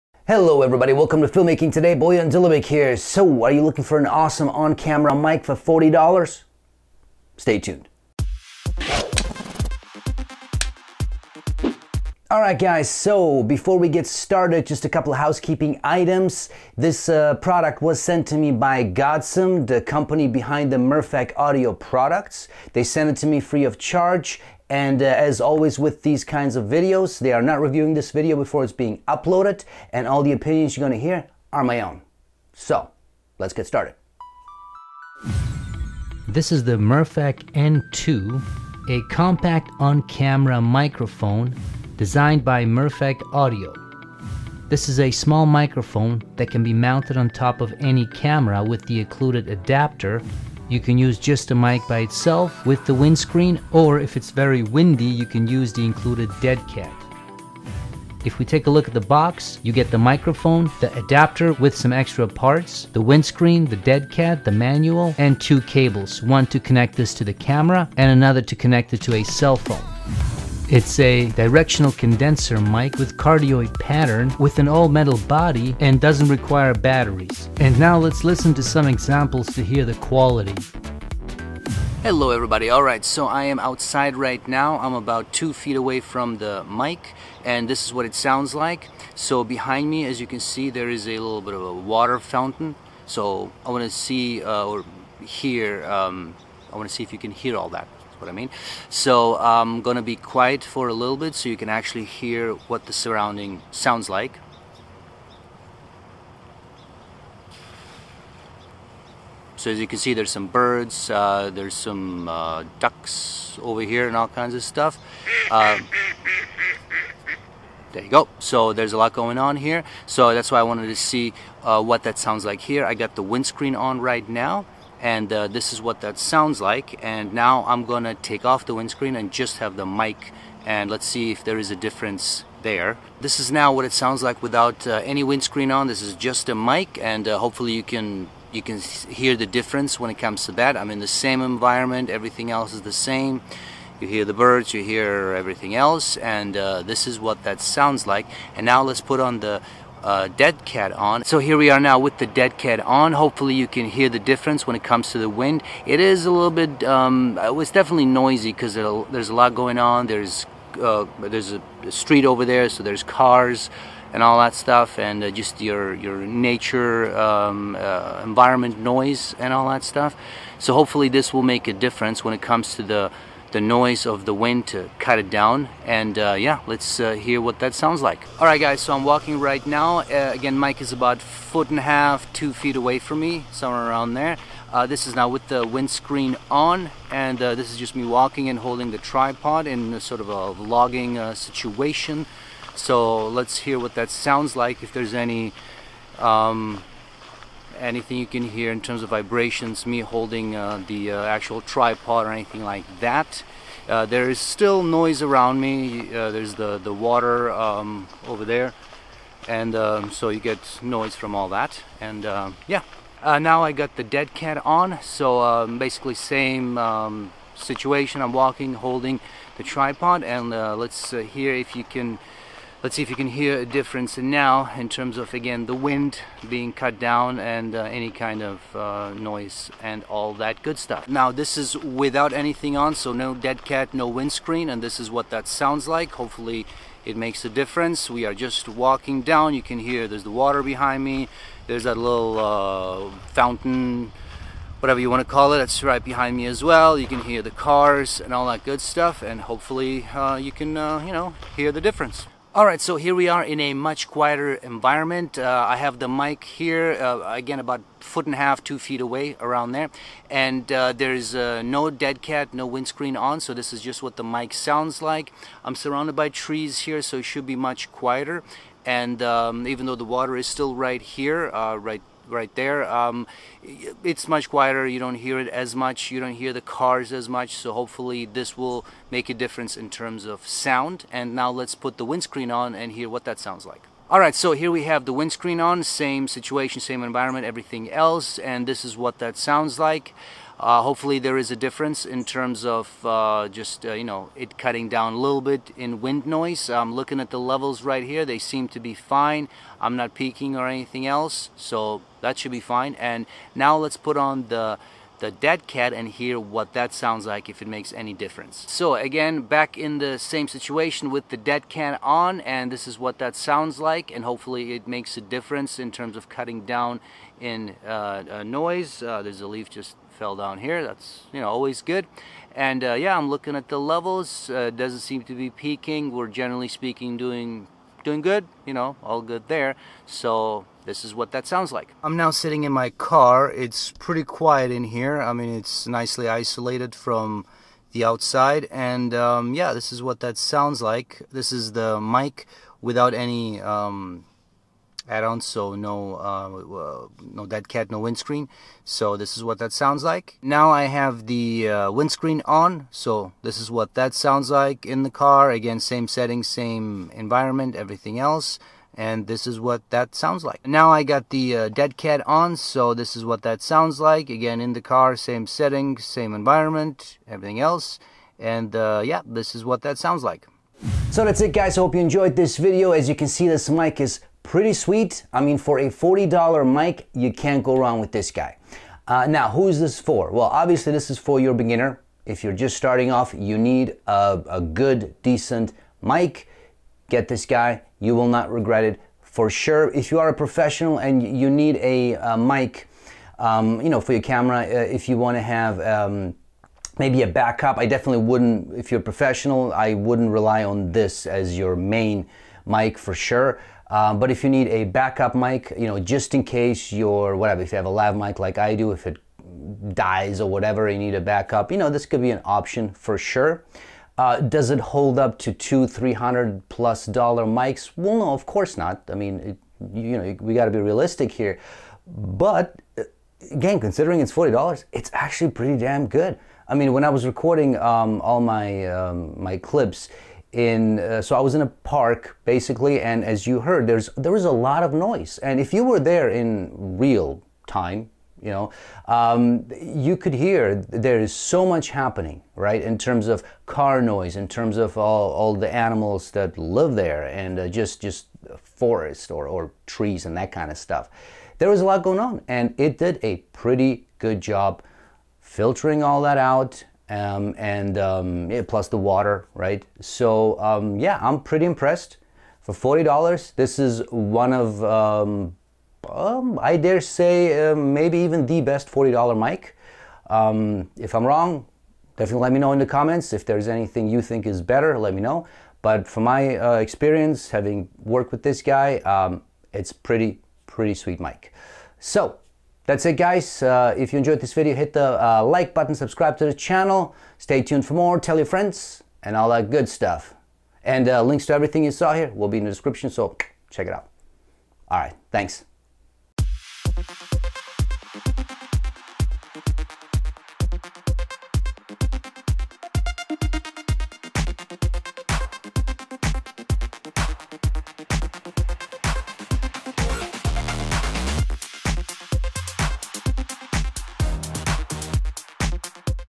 Mirfak N2 Review $40 On-Camera Microphone
This is a detailed review about Mirfak N2 ，a $40 On-Camera microphone.